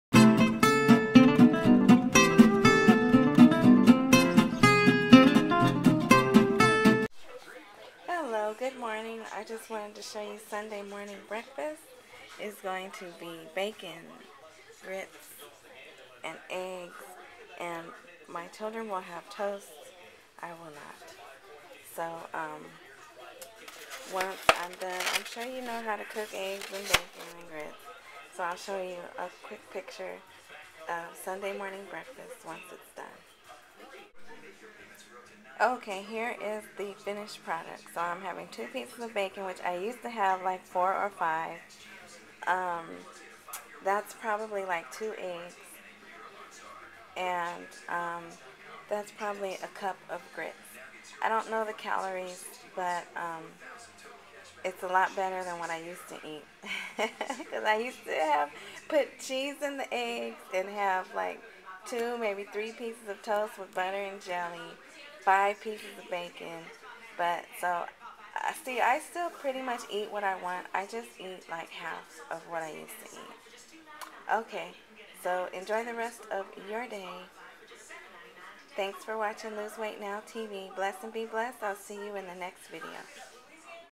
Sunday Morning Breakfast
Sunday morning breakfast at my house.  I don't know the calorie count but this is better than what I used to eat.